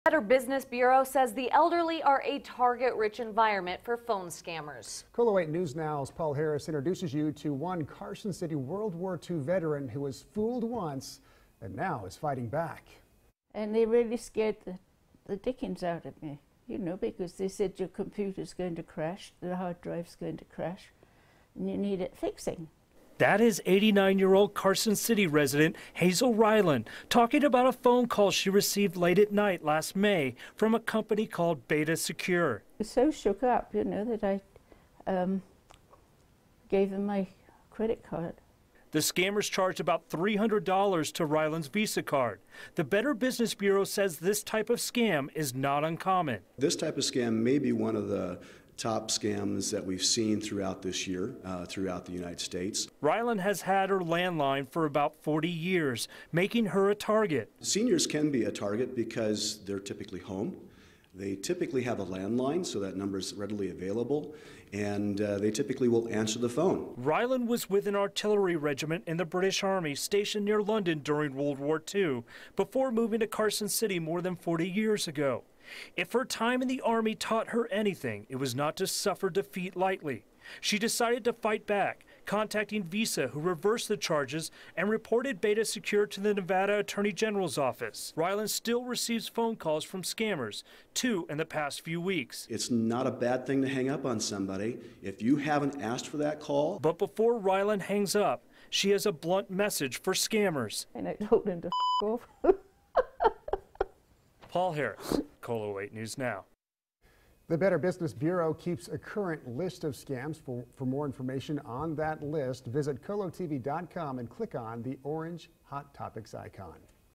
World War II Veteran Fights Phone Scammers
CARSON CITY, NV -- The Better Business Bureau says the elderly are a target-rich environment for phone scammers.

“They really scared the dickens out of me because they said your computer is going to crash, the hard drive is going to crash and you need it (fixed),” said Hazel Ryland, World War II veteran.

The 89-year-old Carson City resident received a late night phone call last May from a company called “Beta Secure.” “I was so shook up I gave them my credit card.”

The scammers charged about $300 to Ryland’s Visa card. The BBB says this type of scam is not uncommon. “This type of scam may be one of the top scams that we have seen this year throughout the United States,” said Tim Johnston, CEO, Northern Nevada BBB.

Ryland has had her land line for 40 years, making her a target.

“Seniors can be a target,” said Johnston. “They are typically home, they typically have a land line so that number is readily available and they typically will answer the phone.”

Ryland was with an artillery regiment in the British Army stationed near London during World War II before moving to Carson City more than forty years ago. If her time in the Army taught her anything it was not to suffer defeat lightly.

She decided to fight back, contacting Visa, which reversed the charges and reported Beta Secure to the Nevada Attorney General’s office. Ryland still receives phone calls from scammers, two in the last few weeks.

“It is not a bad thing to hang up on somebody if you haven't asked for that call,” said Johnston.

But before Ryland hangs up she has a blunt message for them.

“I tell them to (expletive) off.”